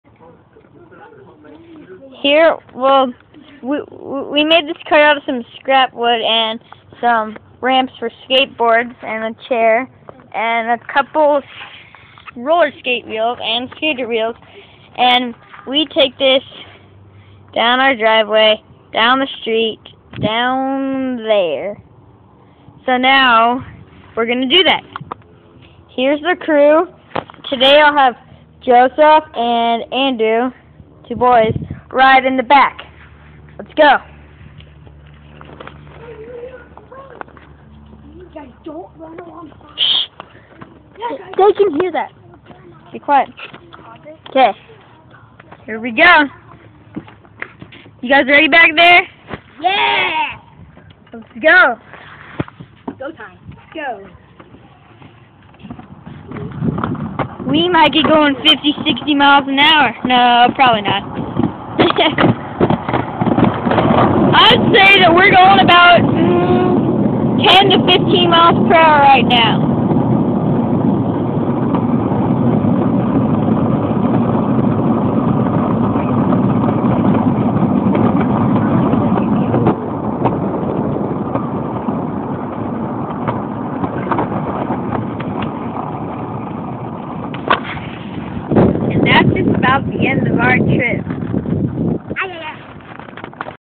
downhill kart 222
Rolling downhill on a homemade wooden cart!!!